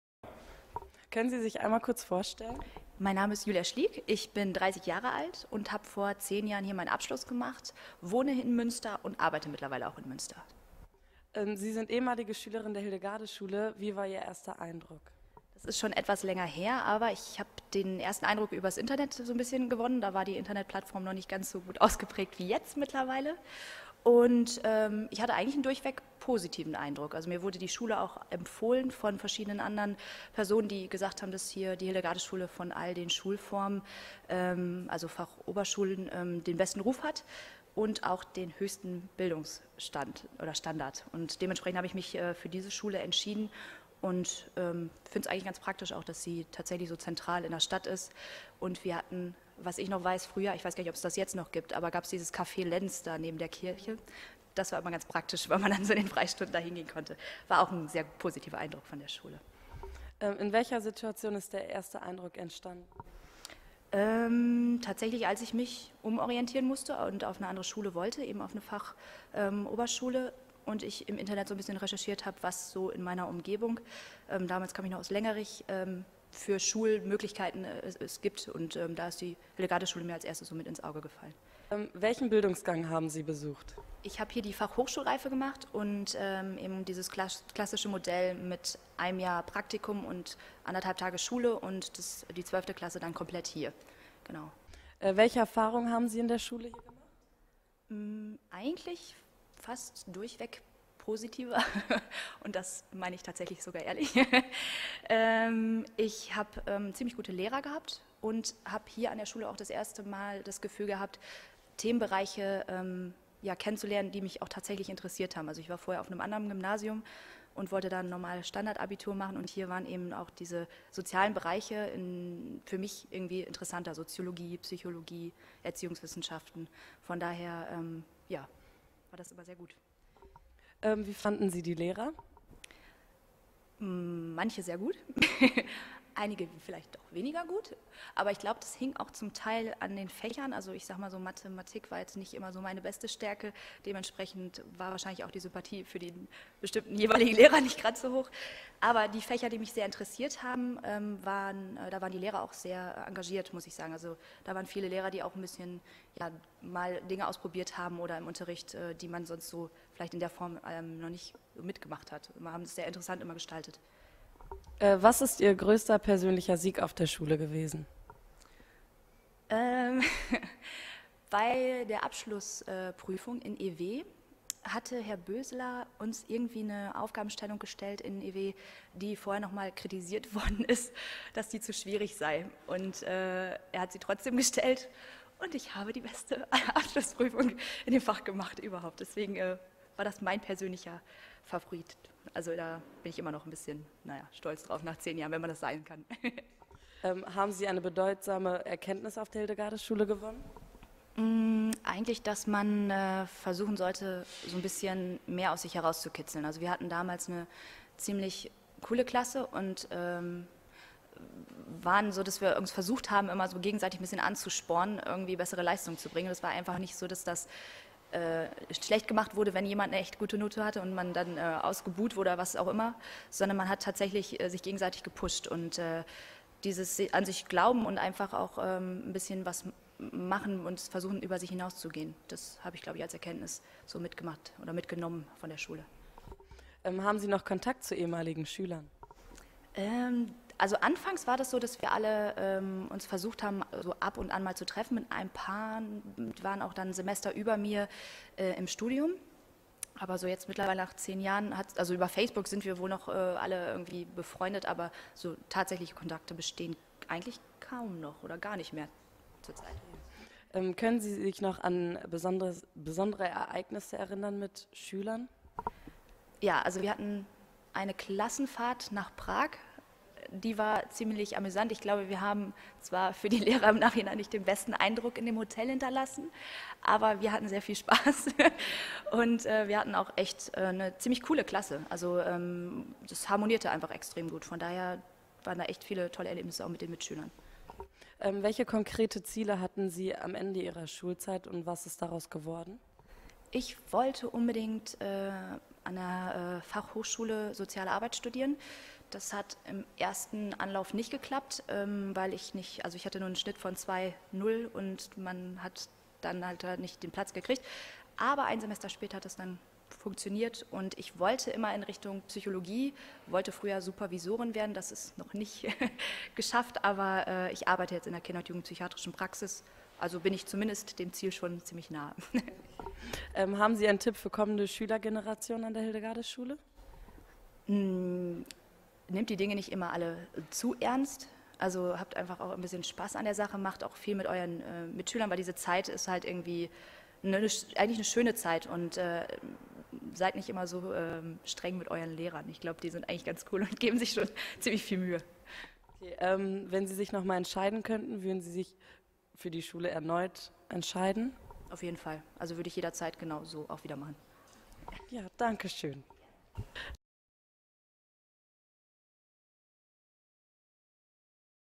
Fachabitur an der Hildegardisschule Münster
Erfahrungen einer ehemaligen Schülerin der Hildegardisschule Münster